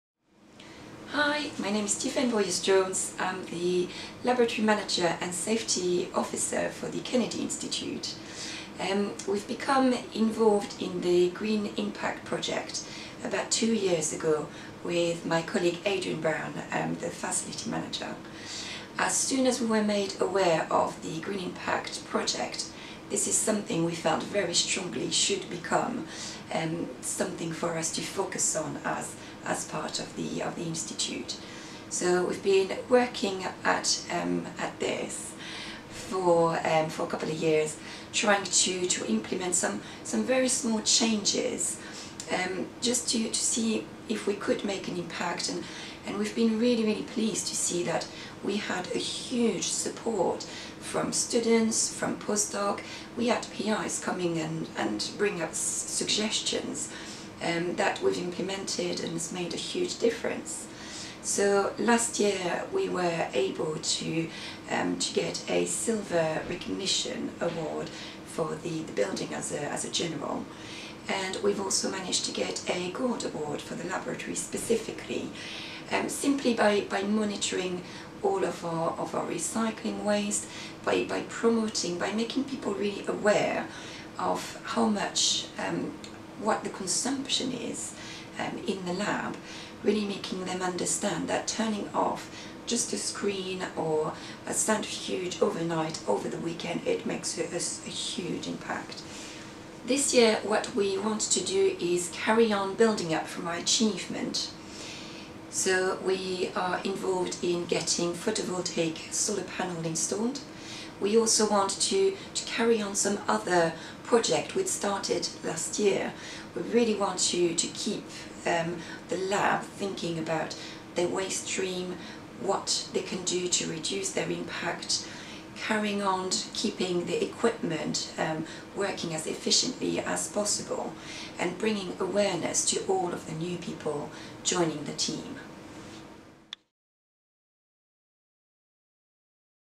Making an impact in more ways than one
The Kennedy Institute has won not one but two Green Impact scheme awards in the University’s fifth Sustainability Showcase – a Silver Award for the building and a Gold Award in the Labs category.

Tiphaine Bouriez-Jones talks on the direction the Green Impact project is heading to next.